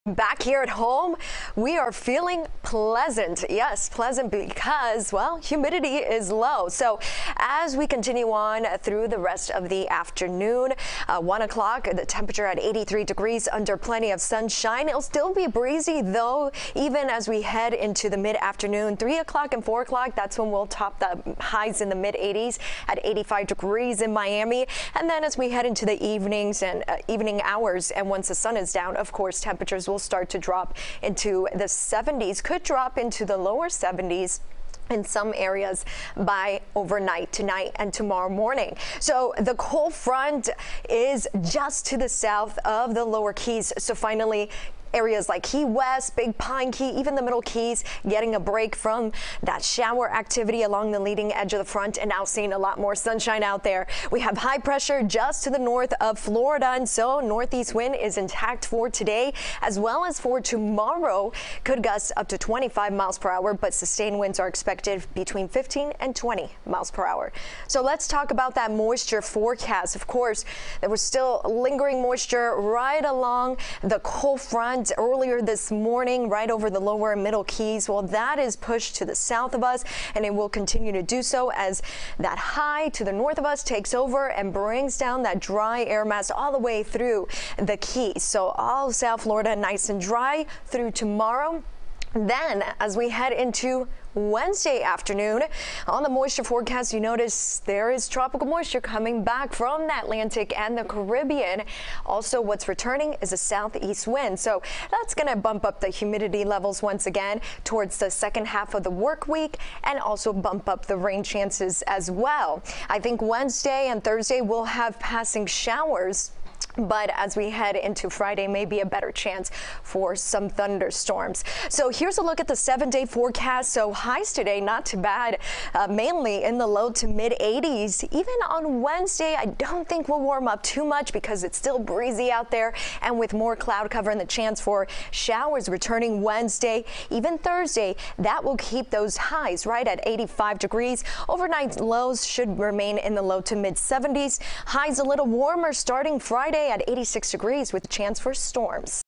CBSMiami.com Weather 10/18/2021 12PM
A mix of sun and clouds with a northeasterly breeze.